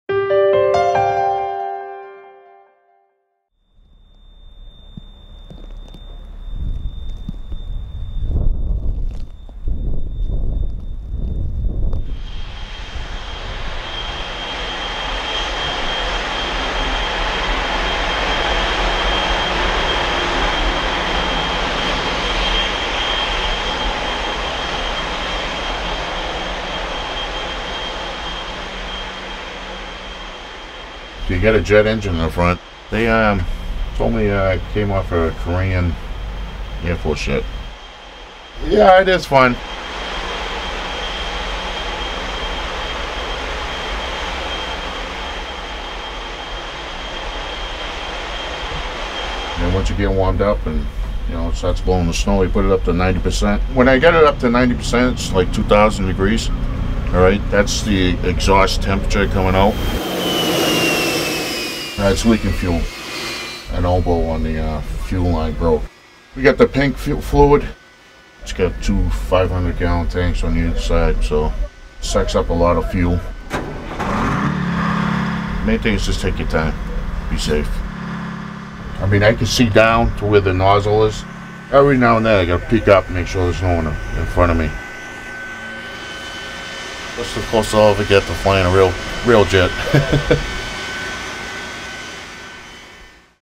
Snowzilla clears the rails on Mattapan Line
(Boston Globe) Jet-powered snowblower clears Mattapan Line so historic trolleys don't fizzle in snow